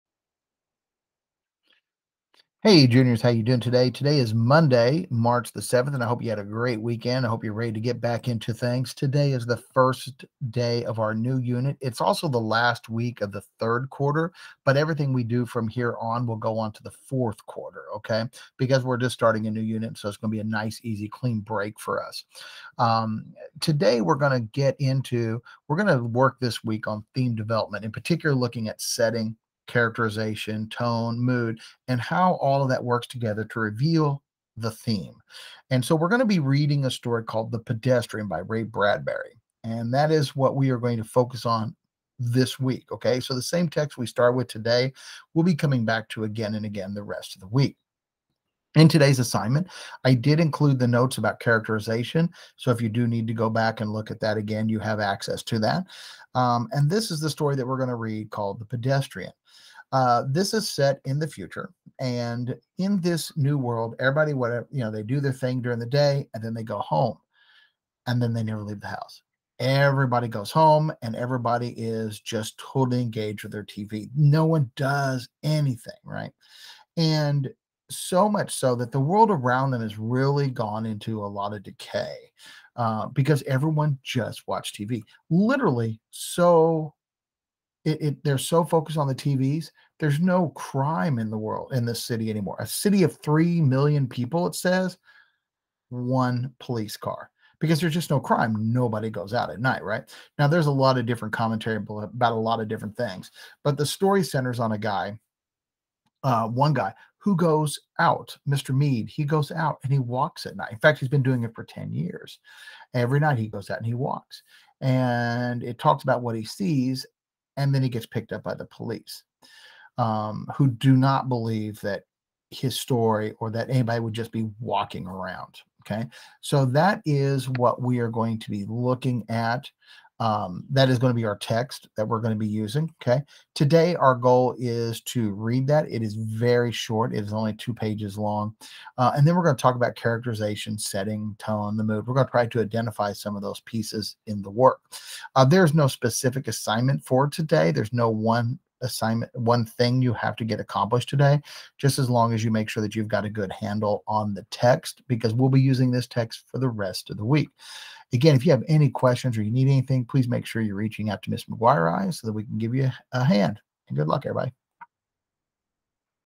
3/7/22 English 11 - Class Notes
This video gives an overview of today's class when we start reading "The Pedestrian."